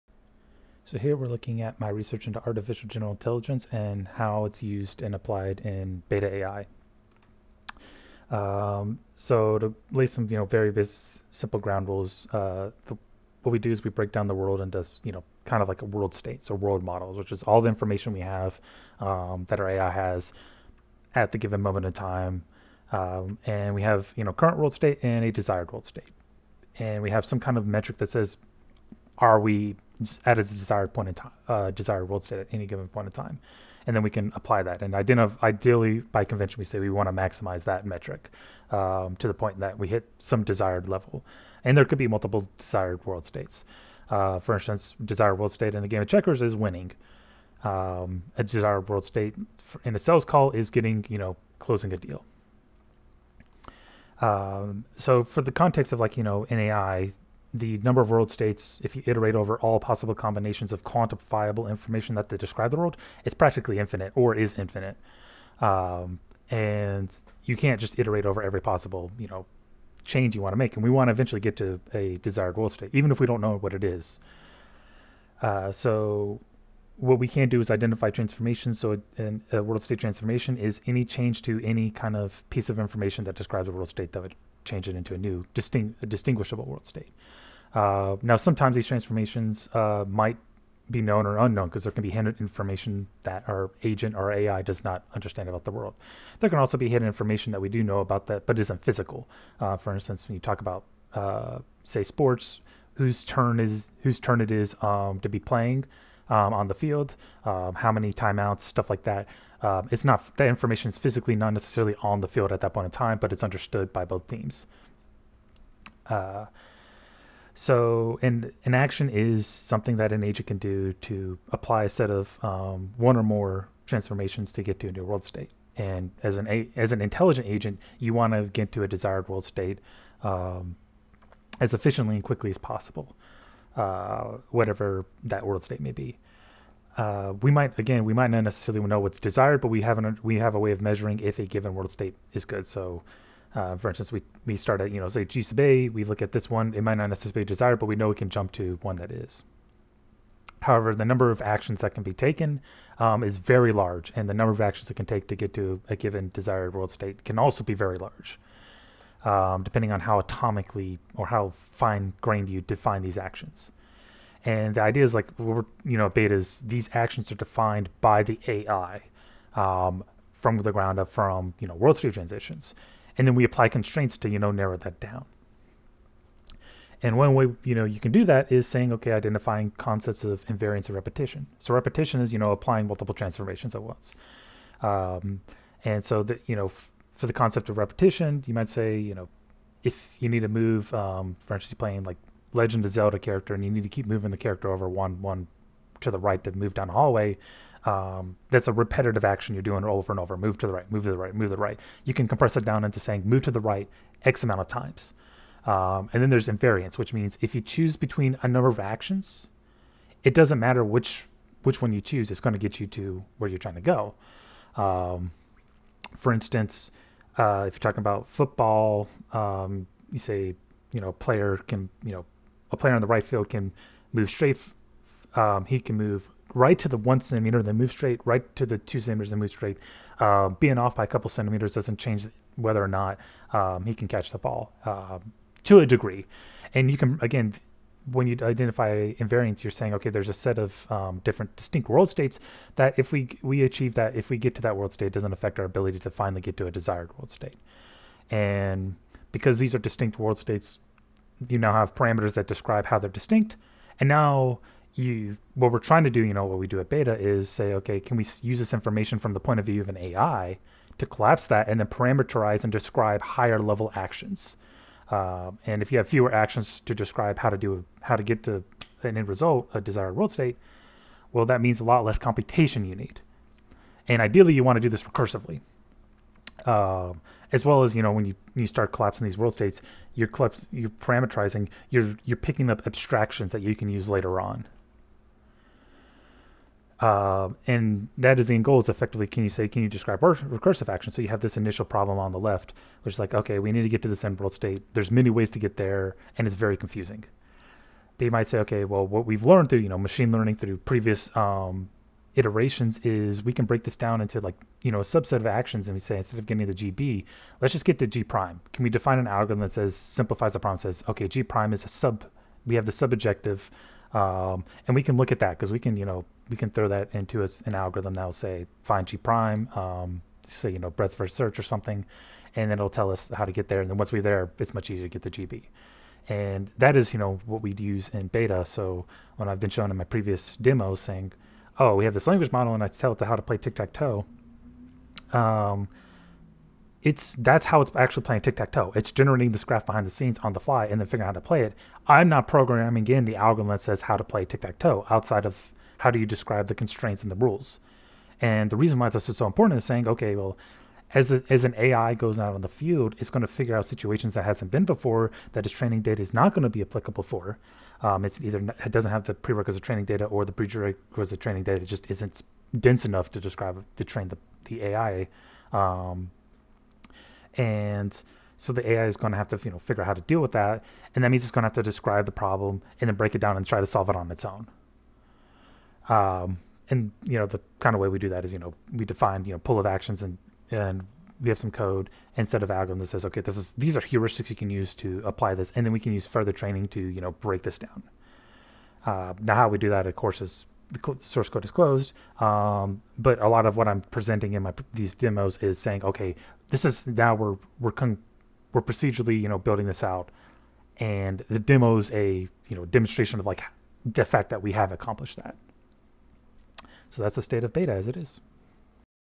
Introduction to Artificial General Intelligence at Baita
Here is a quick 30000 foot overview of how we approach AGI research at Baita. Our research is still ongoing, and all of the demos posted for Baita are showing the progress of that work. The core algorithm we use (the winter tree algorithm) works by taking a nondeterministic finite state machine model of the world, and recursively collapsing transitions into smaller deterministic state models, or statistical models.